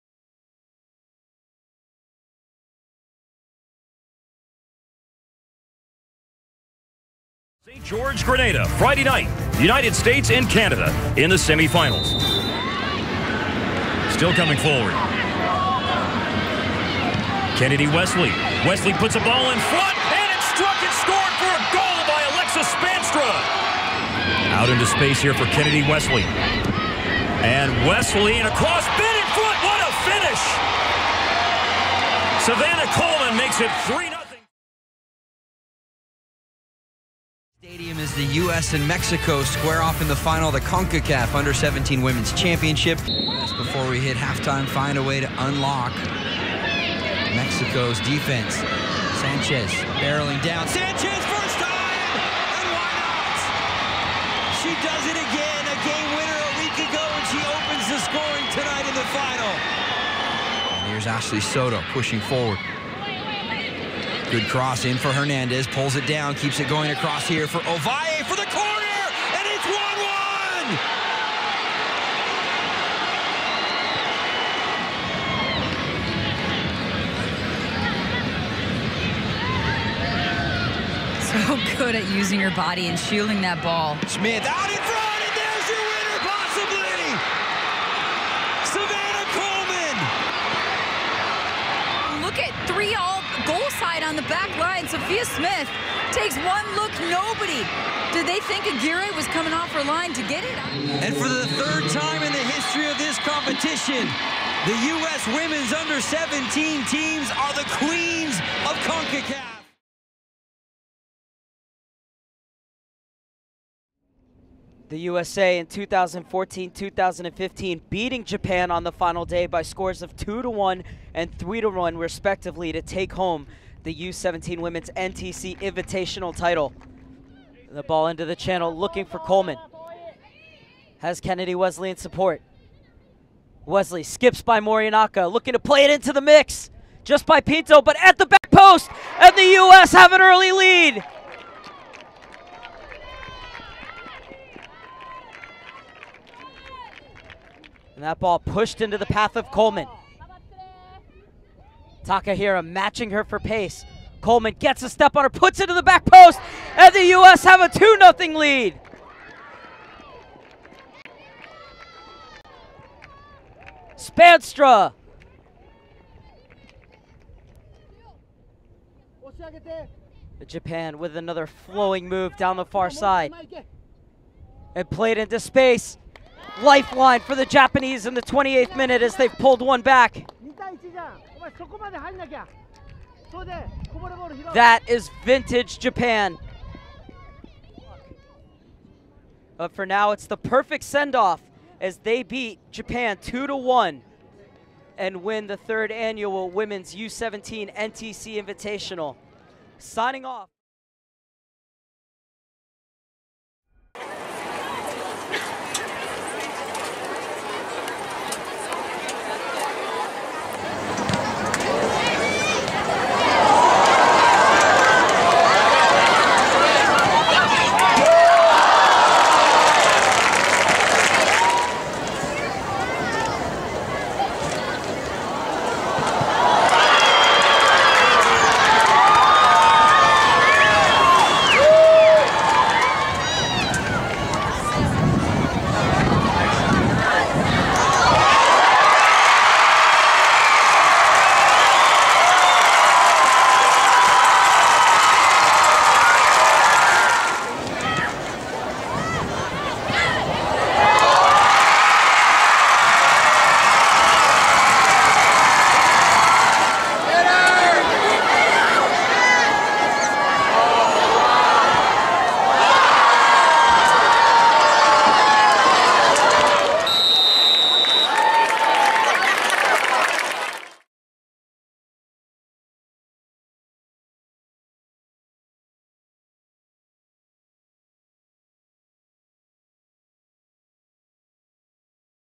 2016 US U17 WNT - Road to the World Cup
The U17 Women's World Cup started today! Follow the US U17 Team as they progress through qualifying towards the biggest tournament of their lives! Ashley Sanchez, Civana Kehlmann, Laurel Ivory and the rest of the girls take on the world!